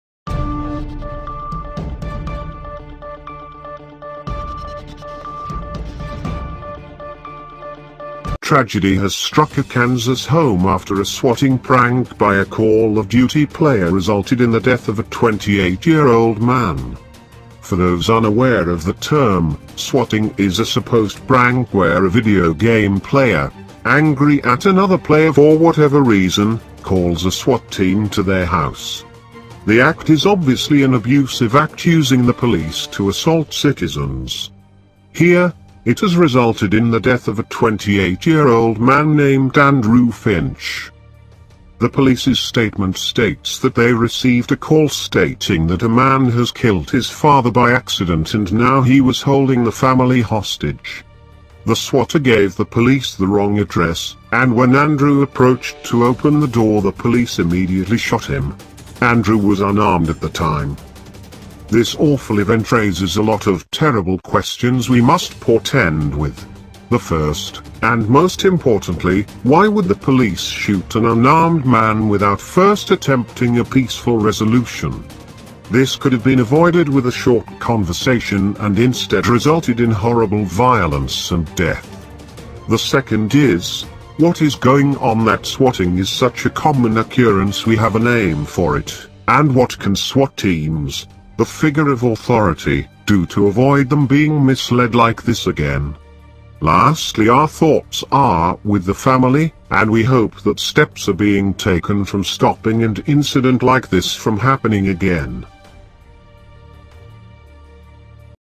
Swatting over Call of Duty game results in deadly police shooting of Kansas man
Death of 28 Years Kansas Man Over Call of Duty Wager